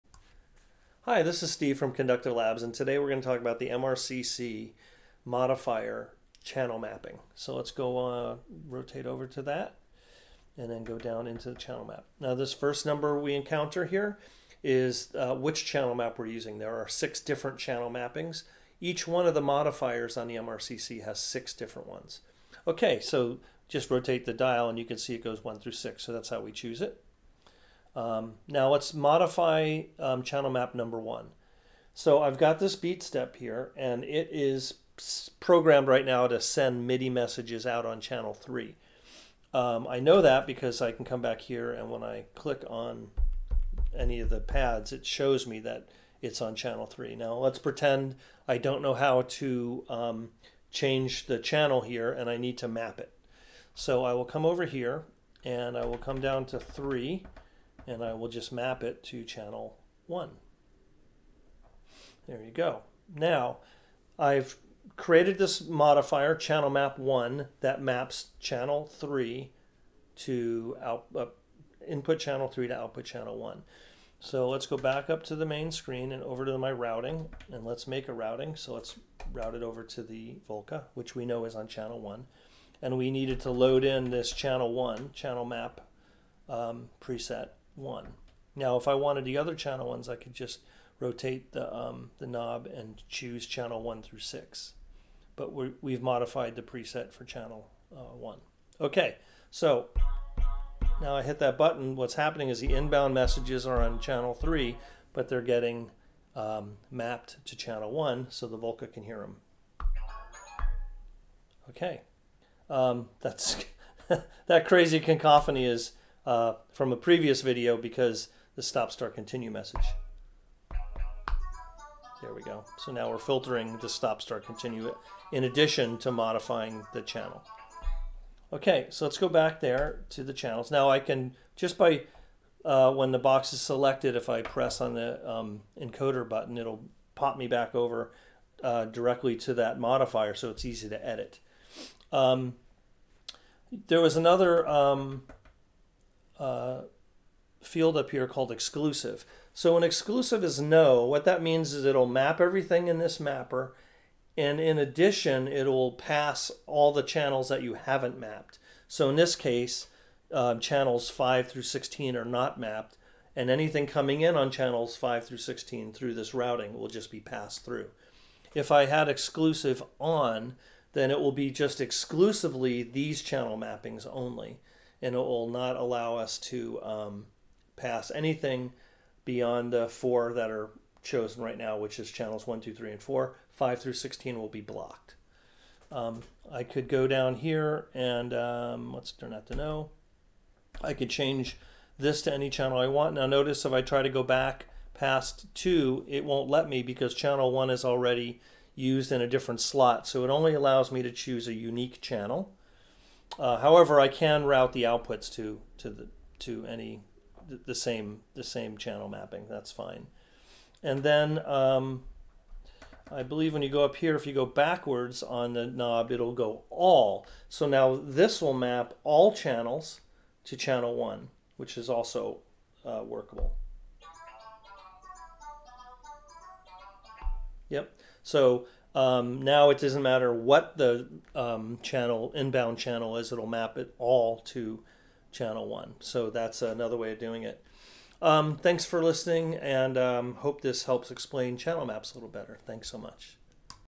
MRCC #11 - Using the Channel Mapping Modifier in a MIDI Routing
Video 11 in the series: How to use the Conductive Labs MRCC - MIDI Router Control Center
Also available at select shops worldwide, including:
Perfect Circuit USA
Patchwerks USA
SchneidersLaden.de
Thomann.de
Juno UK
Signal Sounds UK